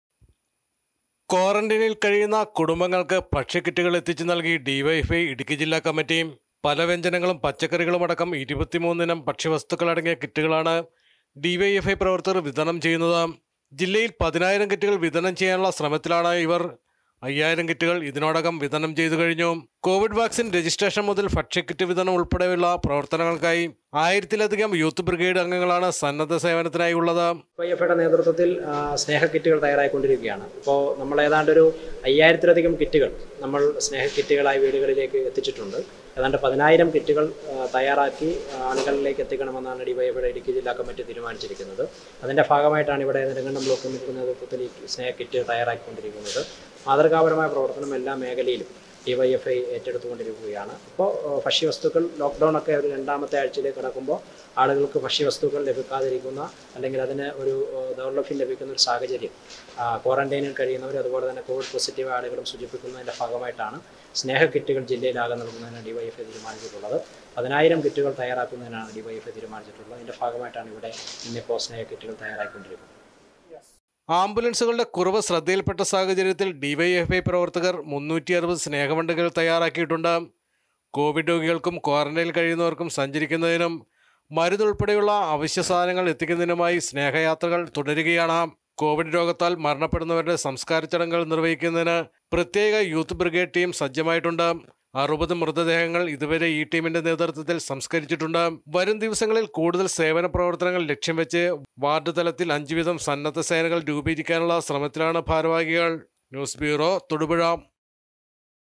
Mediatoday News Thodupuzha 22-05-2021
ക്വാറൻ്റെയ്നിൽ കഴിയുന്ന  കുടുംബങ്ങൾക്ക്  ഭക്ഷ്യ കിറ്റുകൾ എത്തിച്ച് നൽകി ഡിവൈഎഫ്ഐ ഇടുക്കി ജില്ലാ കമ്മിറ്റി.  ഒരു കുടുംബത്തിന് ഒരു മാസത്തേയ്ക്കുള്ള ഭക്ഷ്യ  വസ്തുക്കളാണ് ഡിവൈഎഫ്ഐ പ്രവർത്തകർ വിതരണം ചെയ്യുന്നത്.